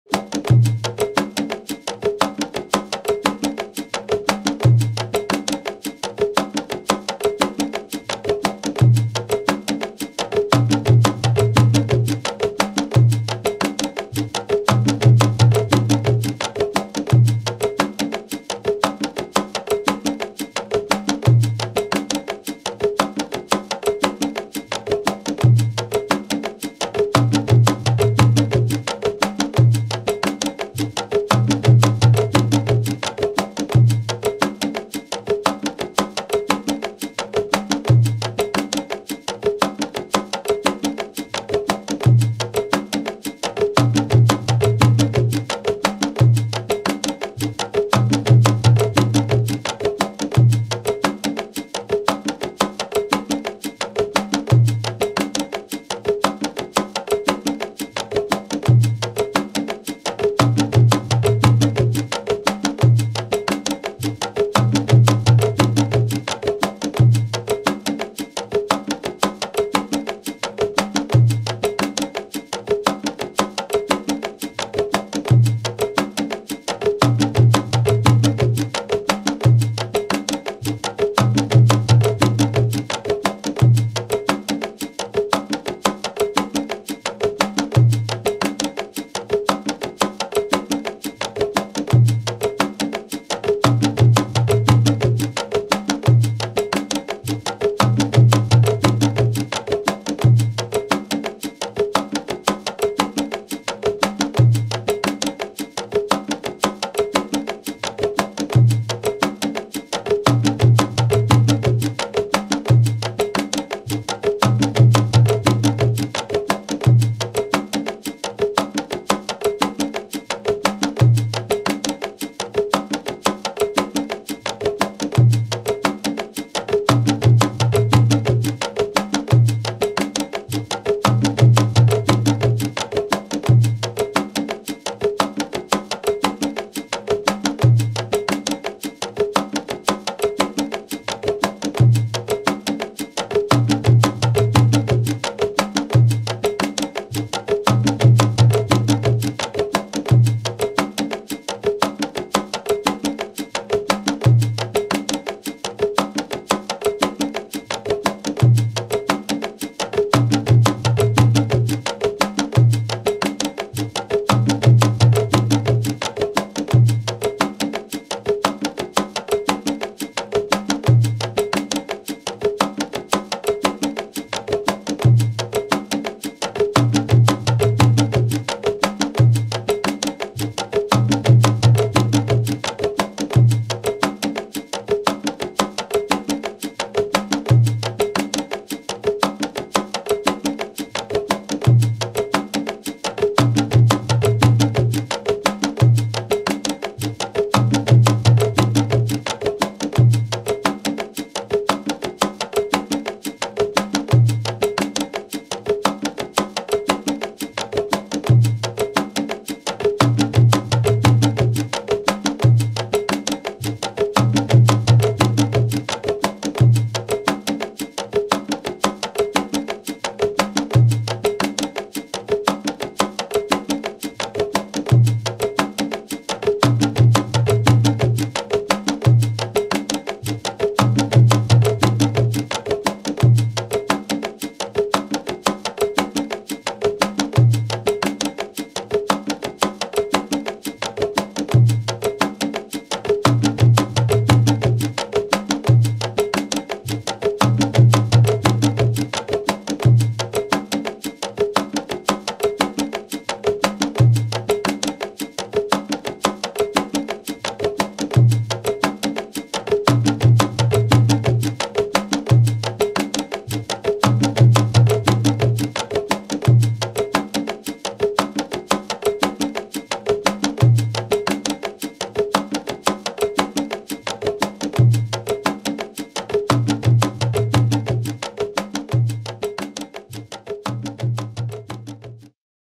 Base TAMBORES BATÁ | Ritmo Afrocubano | 115.5 bpm
PERCUSSION SOUNDS

BASE TAMBORES BATÁ   (RUMBA CUBANA)
115.5 BPM

BASE DE PERCUSIÓN:  GUIRO, SHEKERE, CAMPANA

Bases de percusión para estudio: CONGAS, QUINTO, REQUINTO, CAJON, BAJO, GUITARRA, BAILE

RITMOS DE CUBA, FOLKLORE AFROCUBANO